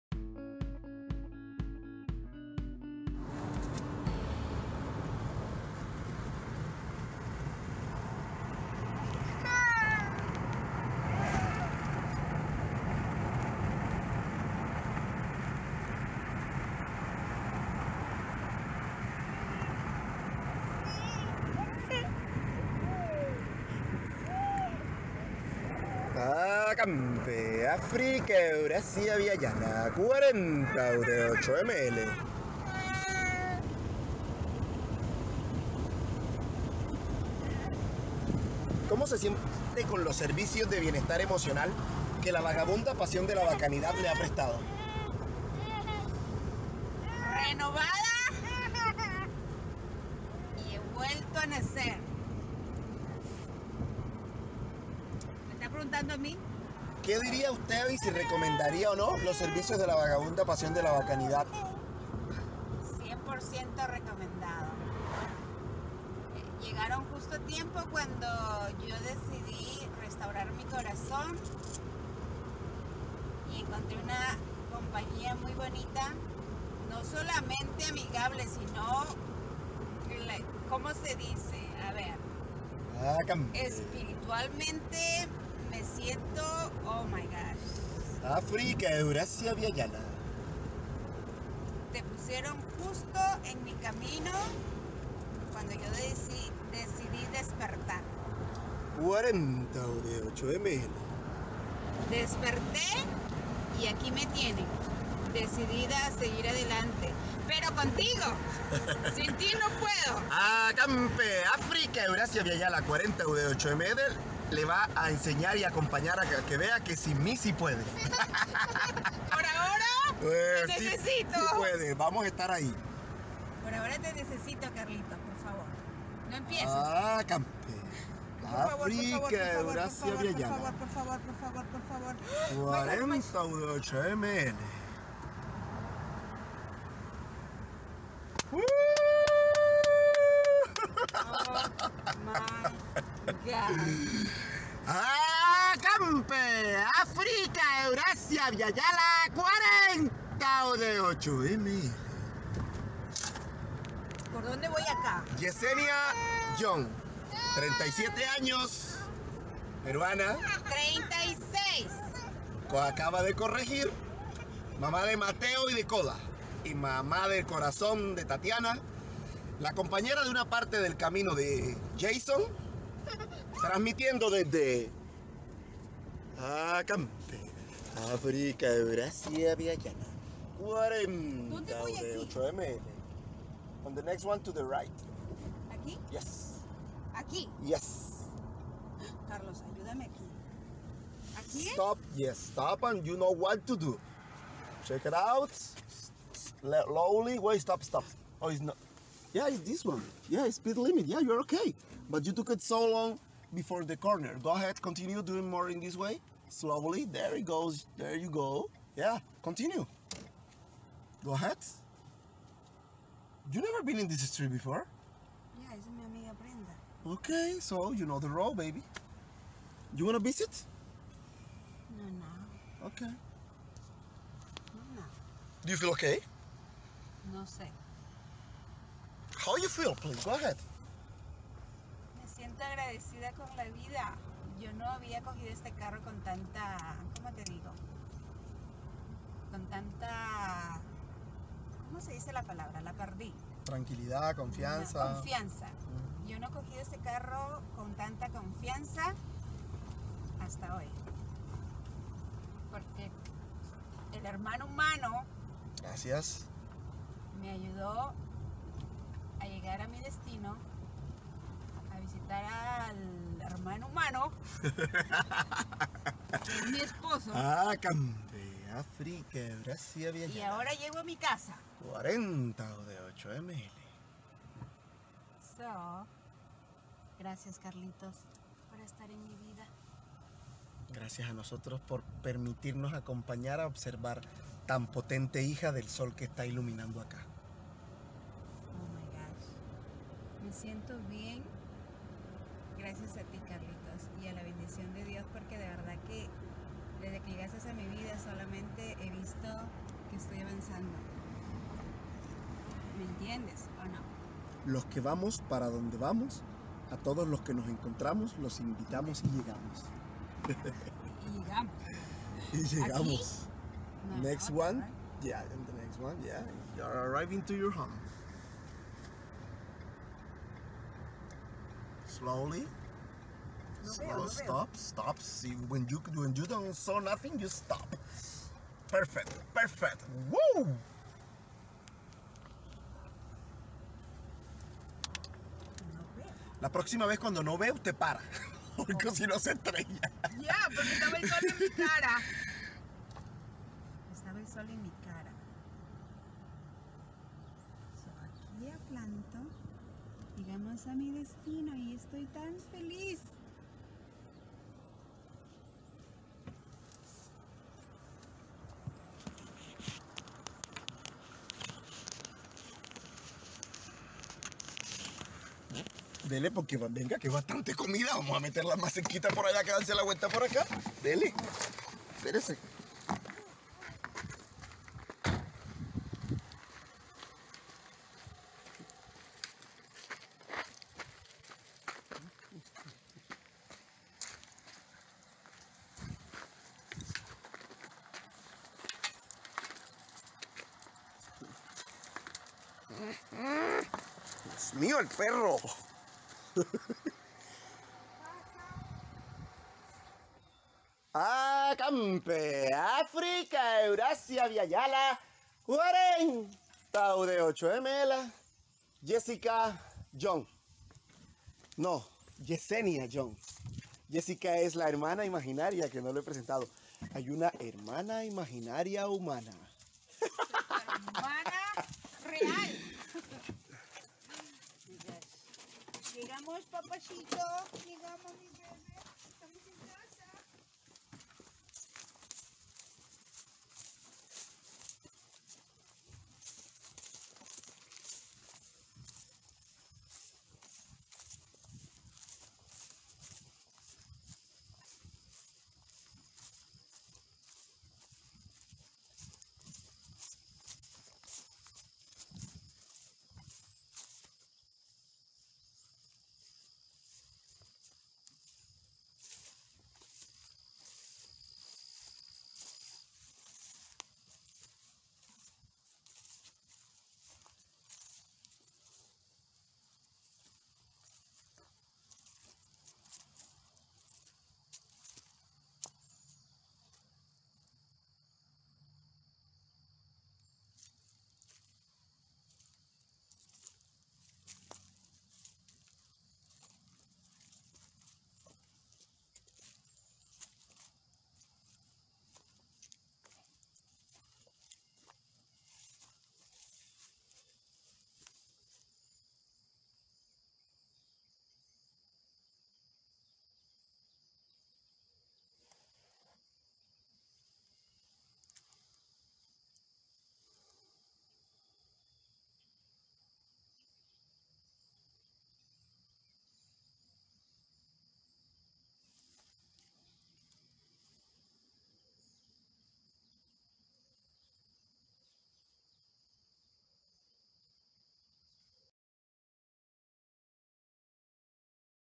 Yesenia John, Ser Viva, Mujer, Mamá, Compañera, Amiga, Hija, Hermana Humana!
La ternura es simultánea a la creación: ella conduciendo la afinación de las cuerdas de la guitarra con la que se entona la canción de la vida suena a su alrededor, y sostenida por creer en el amor, sólo música en armonía retumba cuando ella pasa por cualquier lugar esparciendo algarabía...

Ella Ternura, Ella Alegría, Ella Sostén y Compañía de lo que se acerque a su hembría: Gracias Hermana Humana por permitirse participar del compartir entre los seres por que sí, sin otro son que el compás de las huellas de la creación cuando se decide un andar sin ego, en compañía y con pasión.